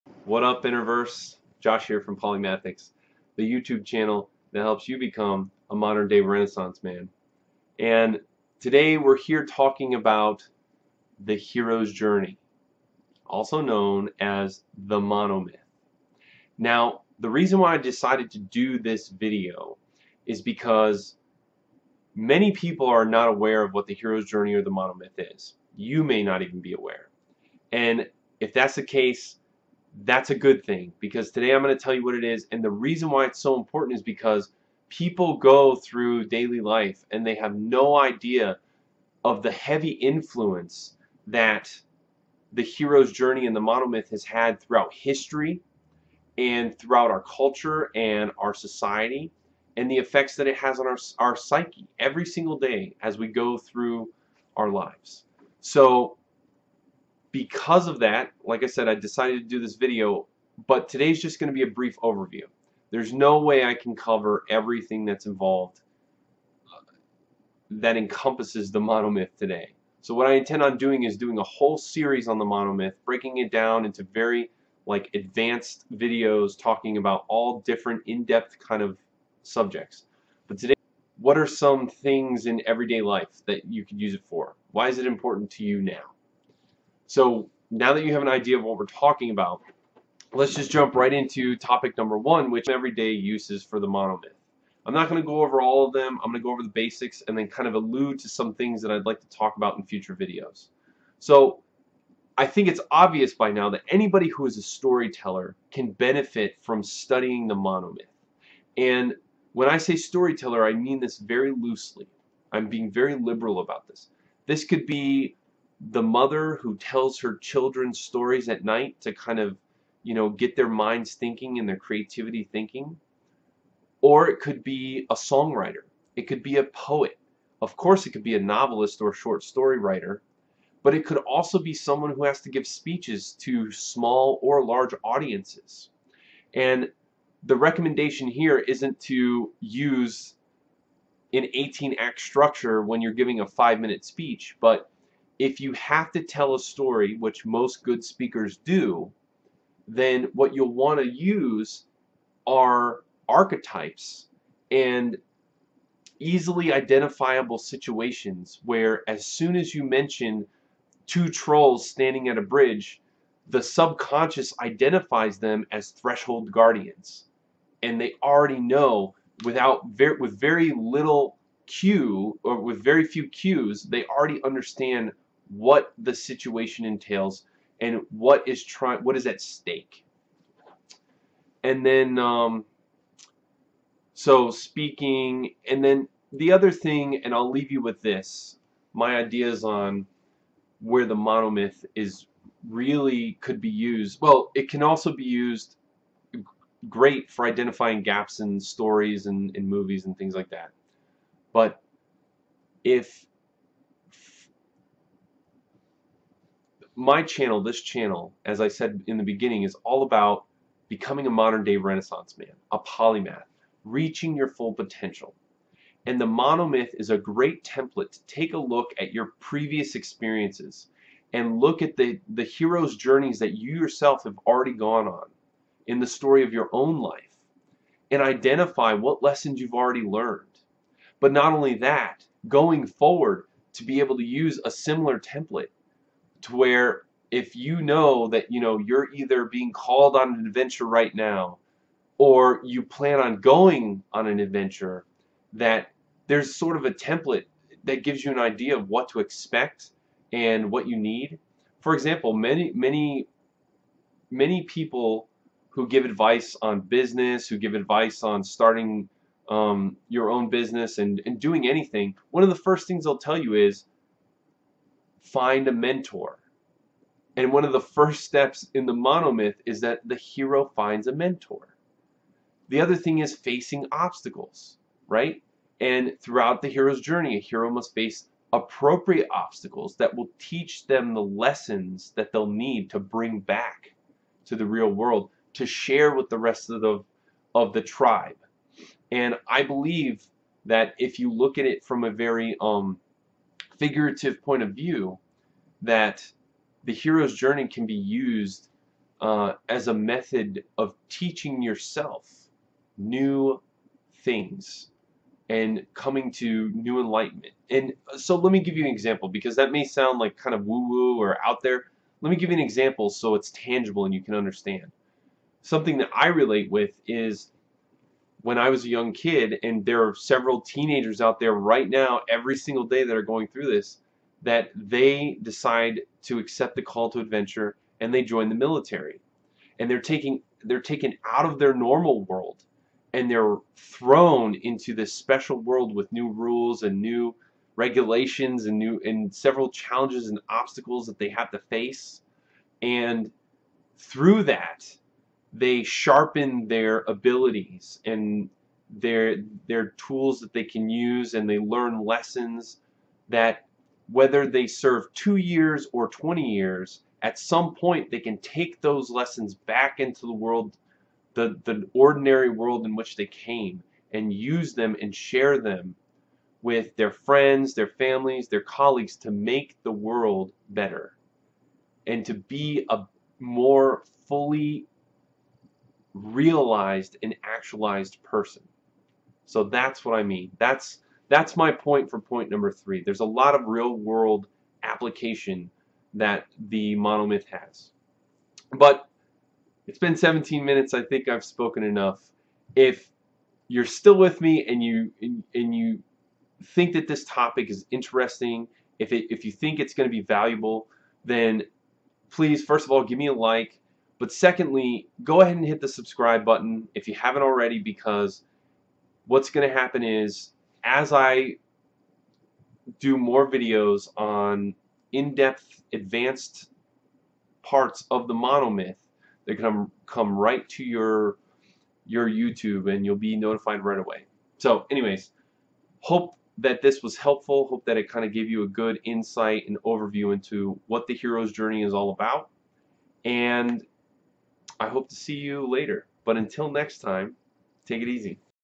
Hero's Journey: What are everyday uses for the monomyth?
In this video Josh explains the basic uses for the monomyth. These include writing and story structure. However, many people find the Hero's Journey as a helpful tool in understanding life experiences and gaining a deeper knowing of their own life’s story.

This post is just to cover the the basics of the Monomyth. Over the course of the next few months Josh intends to provide several detailed explanations on the different aspects of the Hero’s Journey, and analyze more advanced steps in the narrative structure of the monomyth.

And he has a book on military transitions